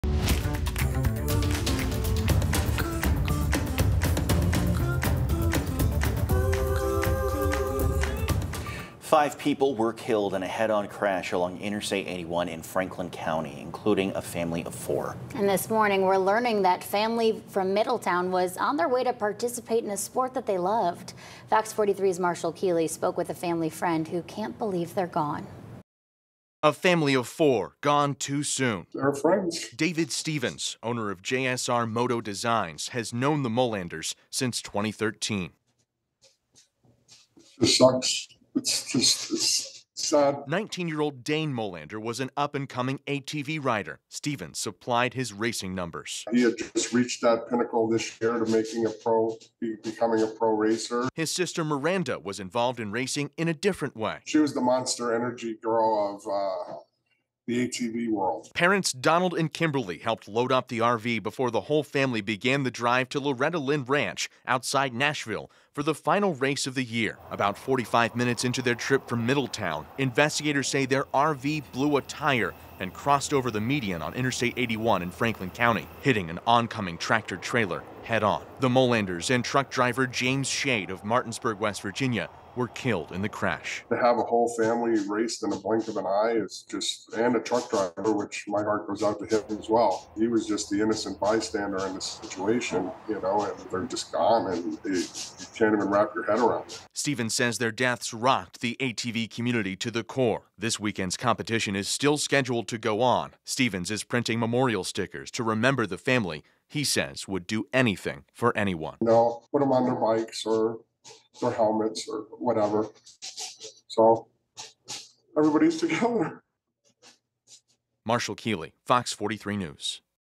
ATV pro, Middletown family killed in head-on crash
A family of four, gone too soon.

"They're our friends," said David Stevens, owner of JSR Moto Designs.

Stevens has known the Molanders since 2013. 19-year-old Dane Molander was an up and coming ATV Rider. Stevens supplied his racing numbers.

"He had just reached that pinnacle this year of becoming a pro racer," Stevens said.

His sister, Miranda, was involved in racing in a different way.

"She was the Monster Energy girl of the ATV world," Stevens recalled.

Parents Donald and Kimberly helped load up the RV before the whole family began the drive to Loretta Lynn Ranch outside Nashville for the final race of the year